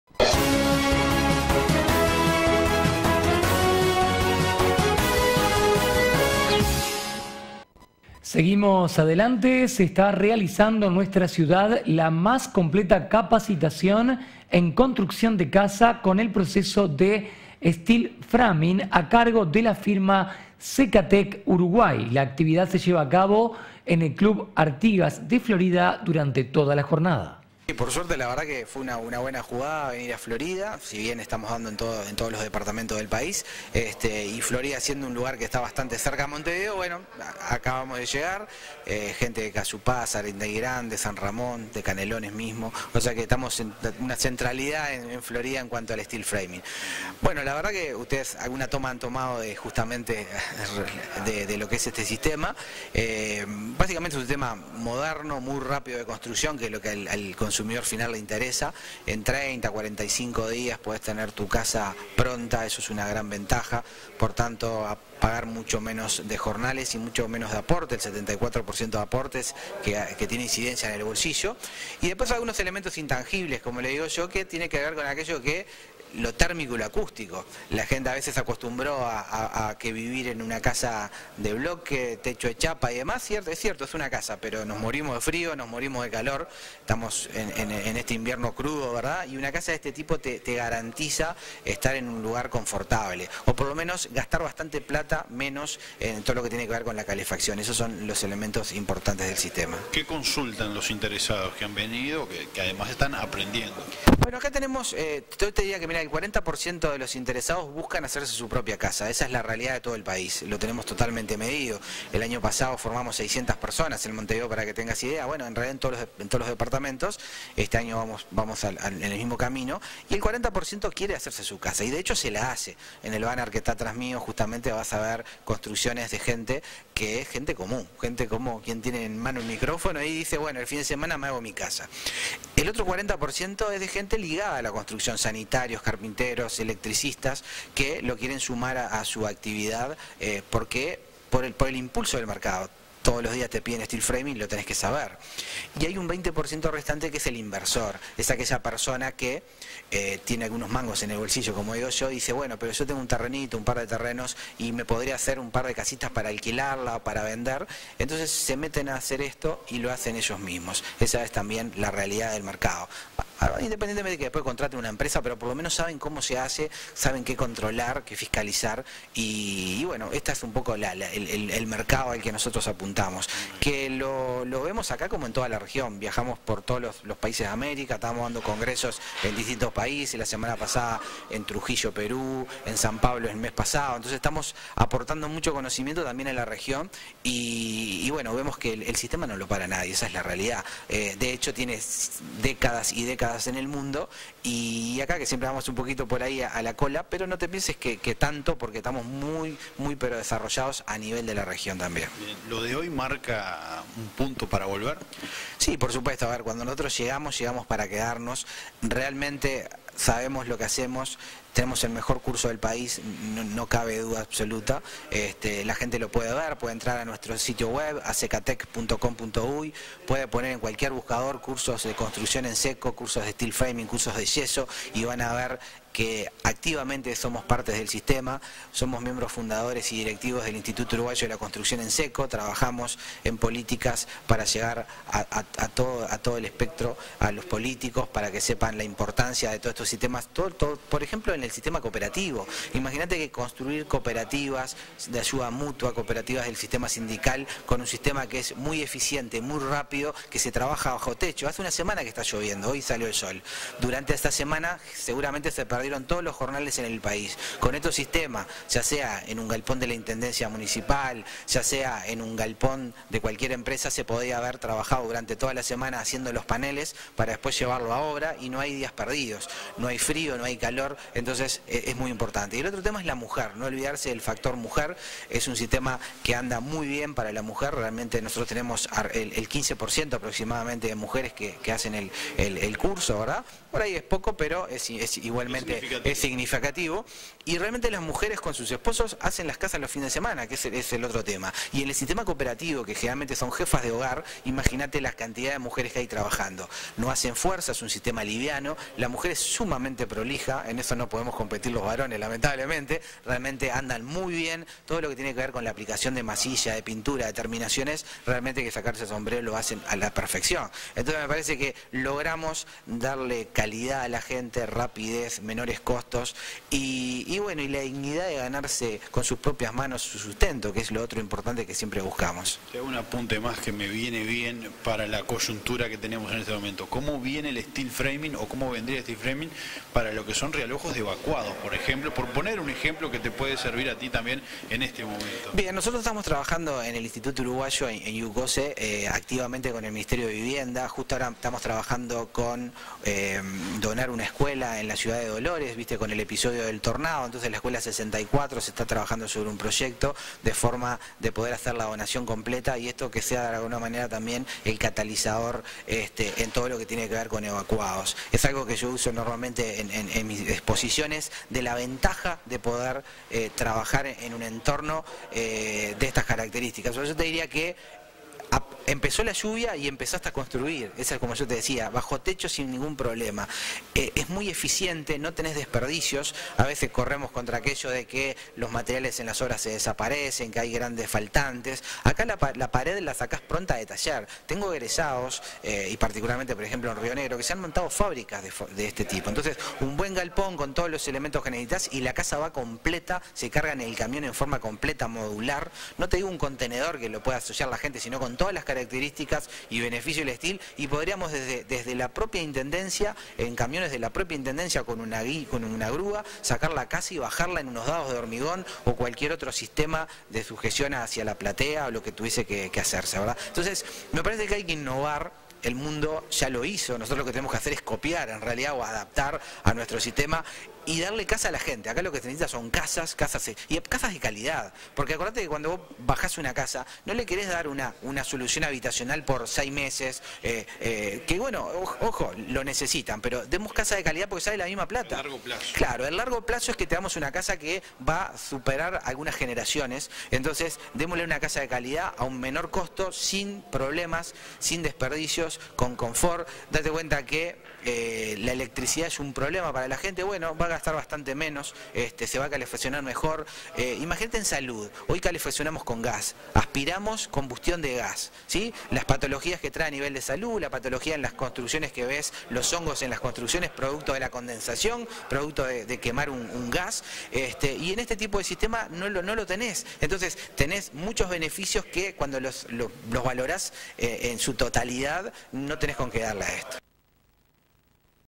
NOTICIERO - Curso de Steel Framing
Un completo curso de instalador de Steel Framing en Florida. Informes Tel. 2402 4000